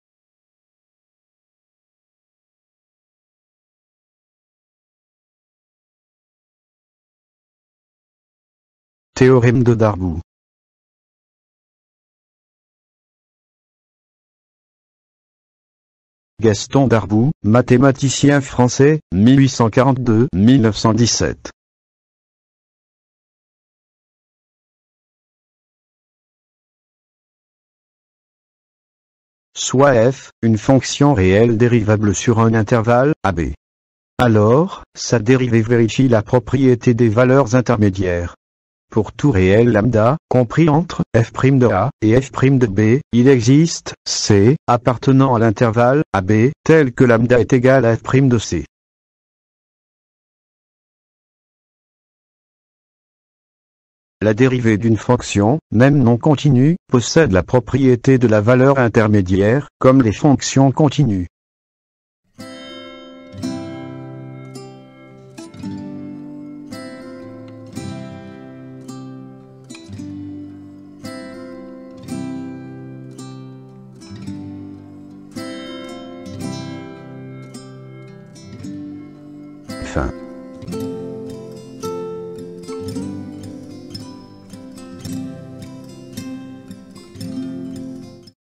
MATHSCLIC : THÉORÈME DE DARBOUX - 1875 -
Théorème d'analyse.
Le théorème des valeurs intermédiaires indique que si une fonction continue sur un intervalle prend deux valeurs m et n, alors elle prend toutes les valeurs intermédiaires entre m et n.
En 1875, Gaston Darboux démontre que toute fonction dérivée vérifie la propriété des valeurs intermédiaires comme les fonctions continues, même si cette dérivée n'est pas continue.